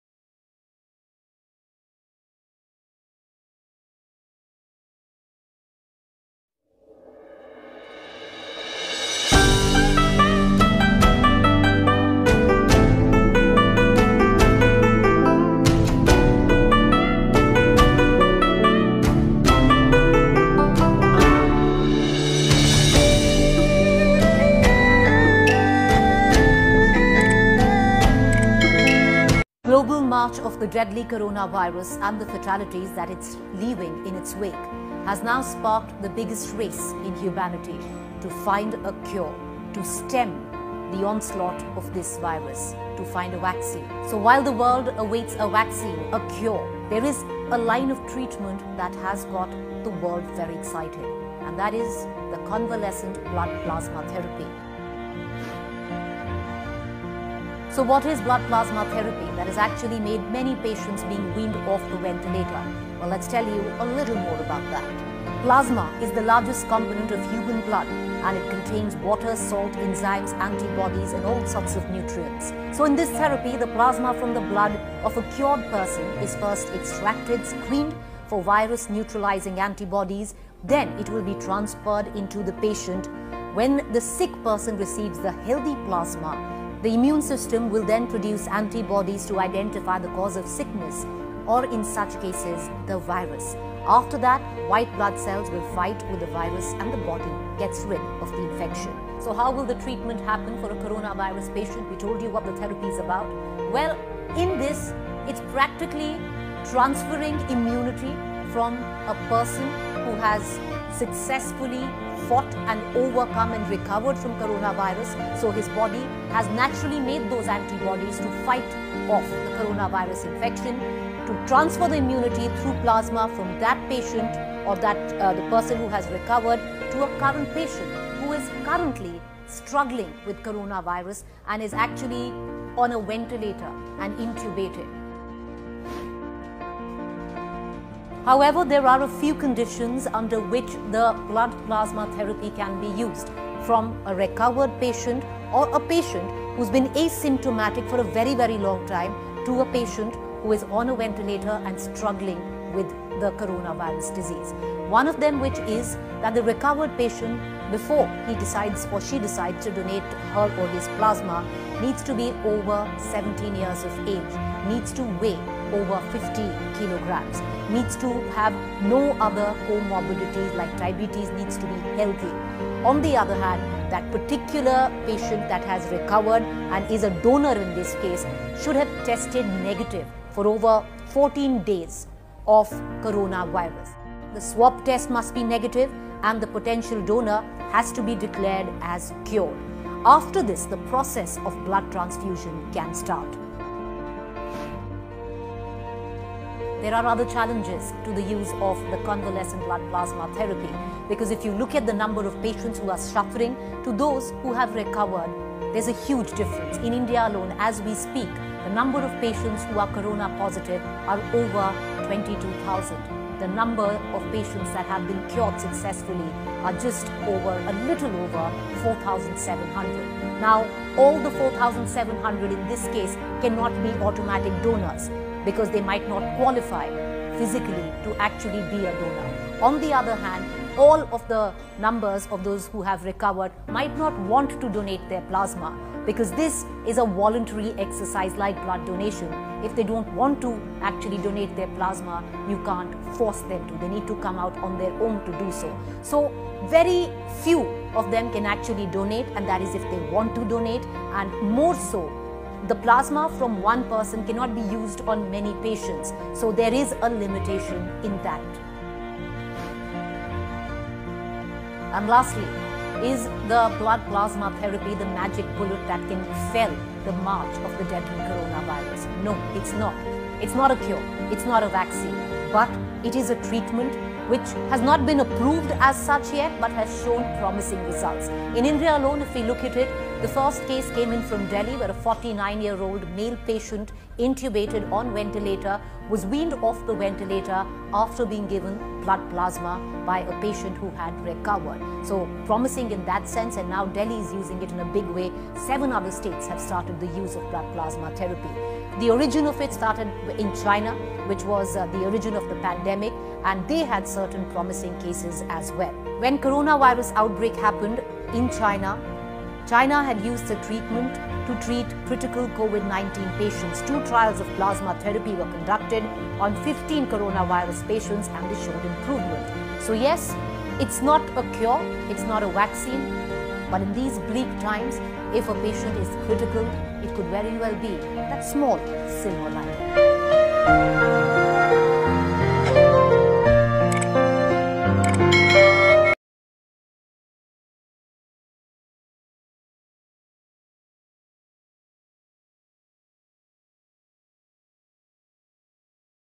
What is Convalescent Plasma Therapy? Understanding of BLOOD PLASMA THERAPY!
This is a report from india and according to the spoke person the plasma therapy is can help to prevent the spread of virus? Watch this for the information and understandings.

We pray to our God Para sa Pangulo natin, Dosctors,  scientist, frontliners etc,,, that there's  hope despite of Pandemic .

This is my Number 8 viral post for   Cure? for covid-19 pandemic.